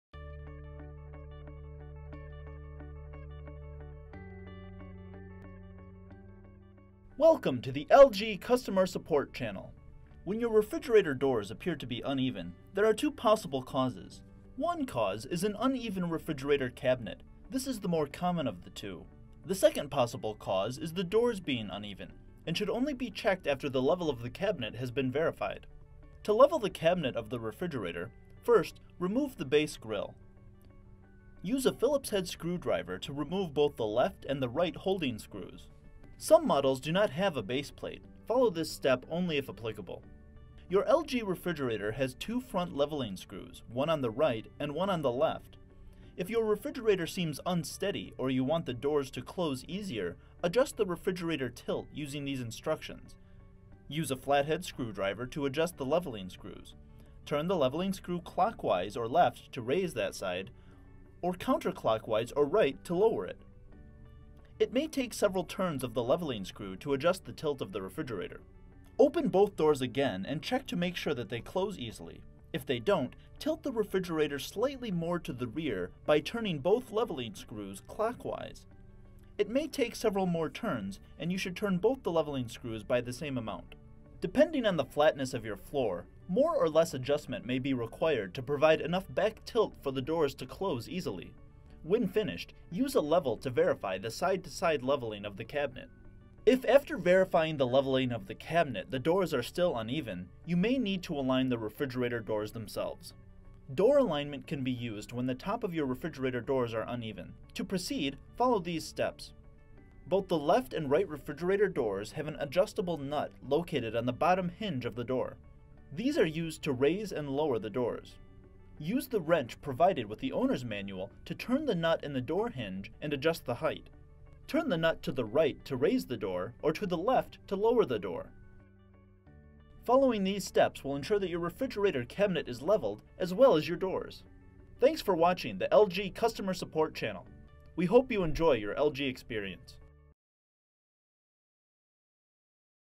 [LG Refrigerators] French Door Leveling & Door Alignment
Learn how to level and align your LG French Door Refrigerator.